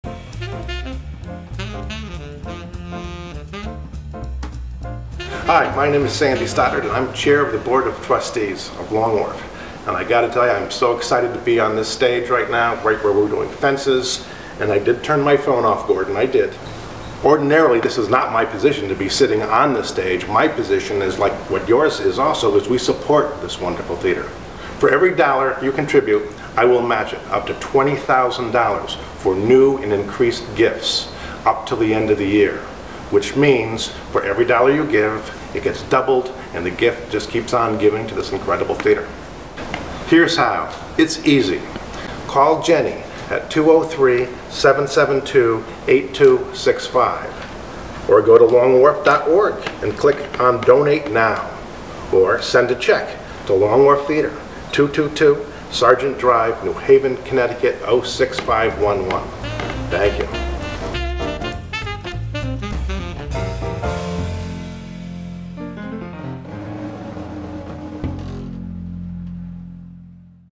The Chairman's Challenge
Sandy Stoddard, Long Wharf Theatre's Chair of the Board of Trustees, makes an appeal from the set of August Wilson's Fences.